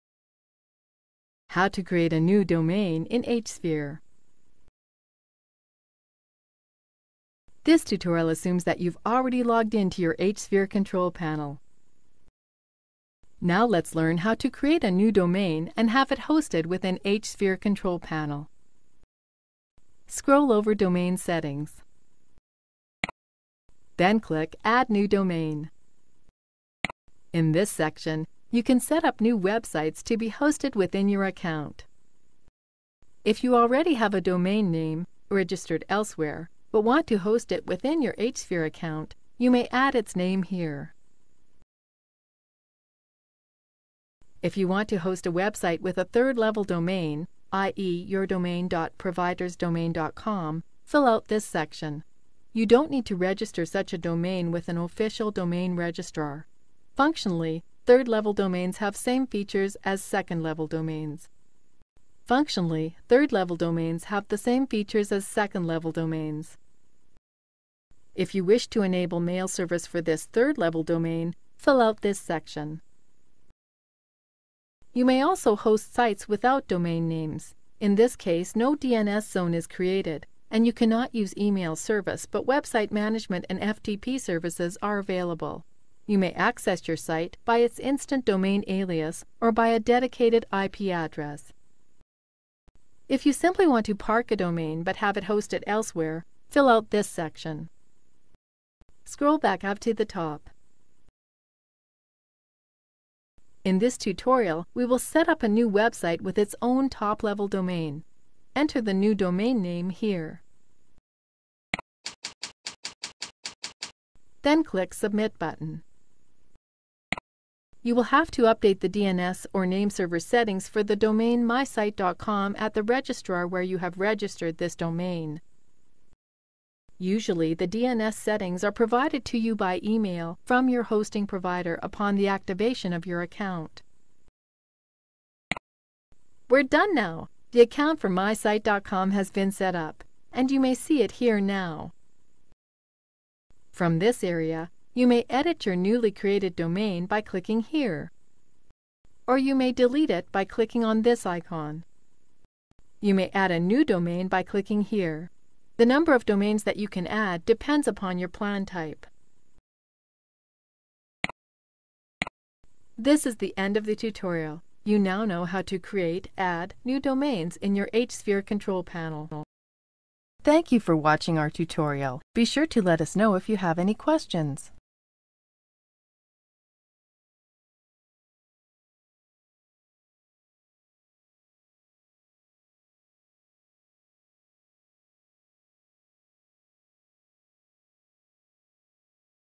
How to create a new domain in H-Sphere
• Log into H-Sphere control panel.
• Scroll over domain settings and click add domain option.
• In this section, you can setup new sites to be hosted within your account.
• If you have a domain name, add its name in textbox to host it. If you want to host a website with third level domain, fill out the respective section. You don't require registering such domain with official domain registrar.
• Sites without domain names can also be hosted. DNS zone is created for you and you can't use email service. Website management and FTP tools are available for such cases.
• DNS settings are provided by host on initiation of a hosting account. Account for site will be setup and can be seen in the listing.
• You can edit or delete accounts by clicking on respective options.